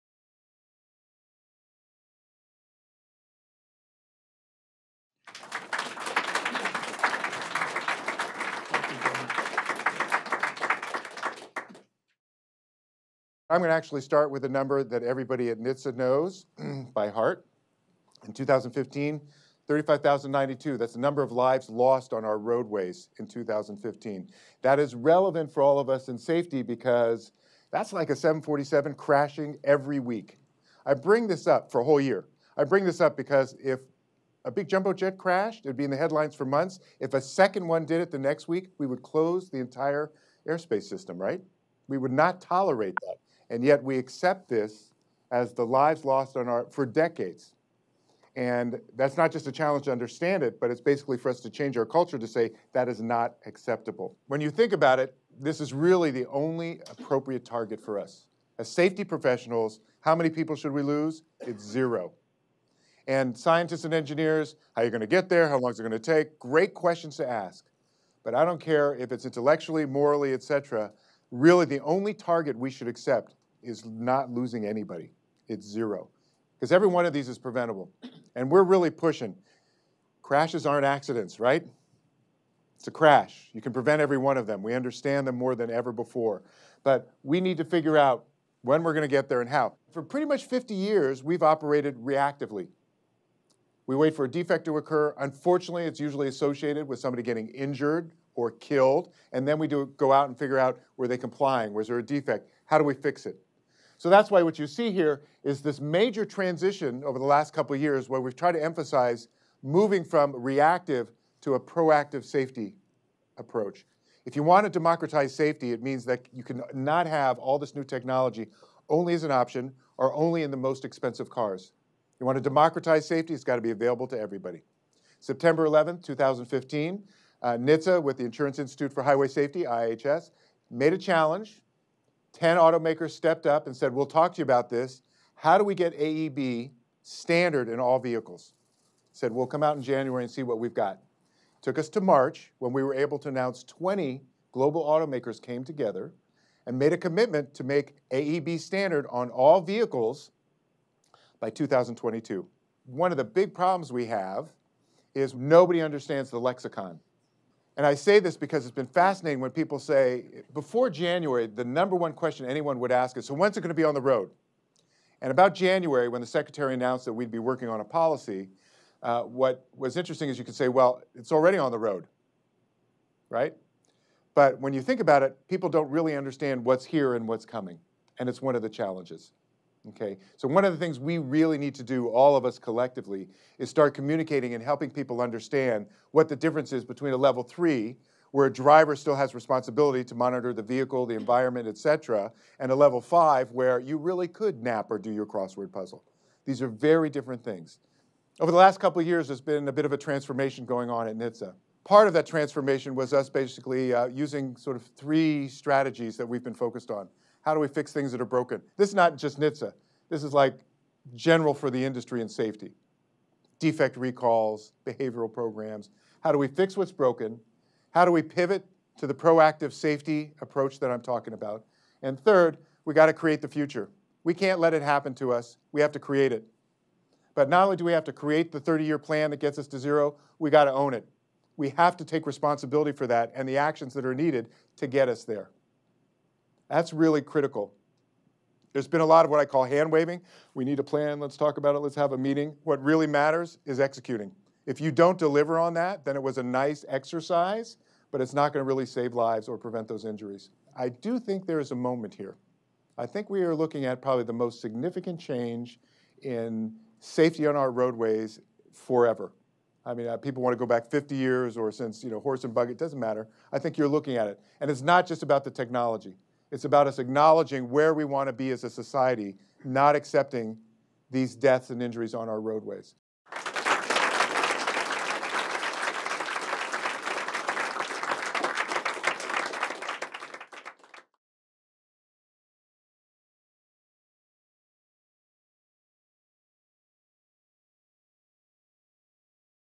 Traveling Three Lanes: On the Road to Zero Vehicle Fatalities
The final presenter in Volpe’s Future of Transportation speaker series was National Highway Traffic Safety Administration (NHTSA) Administrator Mark Rosekind, PhD.

Administrator Rosekind actively pursues NHTSA’s core safety mission through public outreach, technical innovation, enforcement authority, and research into human behavior.

Prior to his position at NHTSA, Administrator Rosekind served on the National Transportation Safety Board (NTSB). He was the on-scene board member for seven major transportation accidents and participated in 50 accident board meetings. Rosekind is also an internationally-recognized expert on human fatigue.

This highlight video captures key excerpts from his talk at Volpe on November 9, 2016.

Please see our Beyond Traffic series page for all speakers and dates